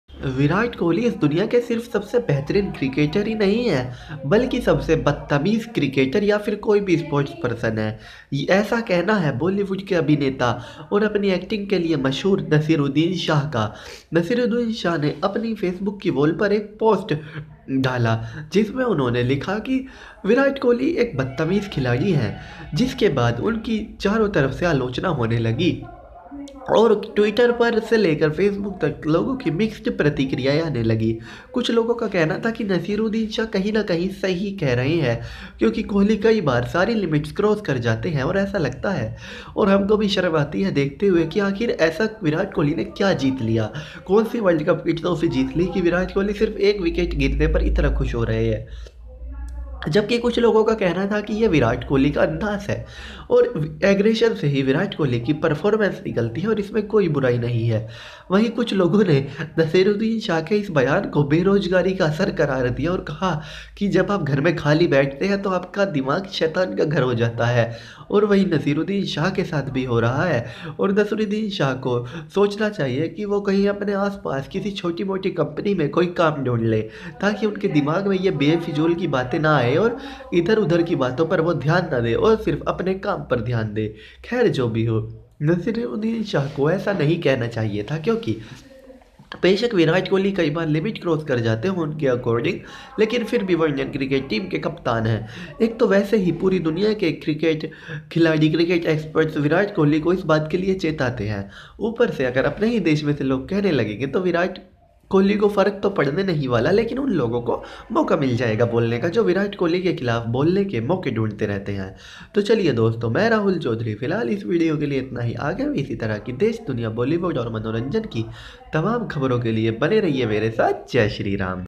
Virat kohli is the worst behaved player in this world - Naseerudin shah | Naseeruddin shah in Virat
naseeruddin shah in his latest Fb post written that Virat kohli is the worst behaved player of this world.